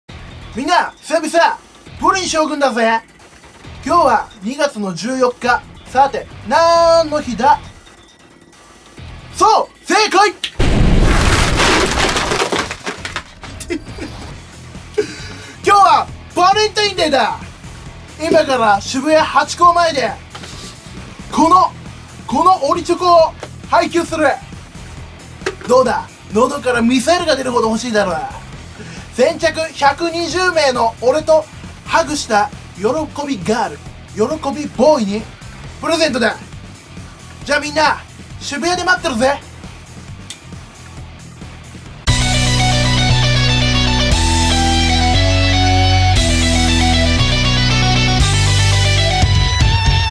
[告知]２月１４日昼１２時〜渋谷にてオリジナルチョコ配給！？ バレンタイン企画
本日、２月１４日お昼１２時〜
渋谷ハチ公付近にて

１２０名にオリチョコを配給する

条件は

①YouTube将軍TV登録
②将軍とハグ

集え！！喜び組！！

※本命チョコまってるで💖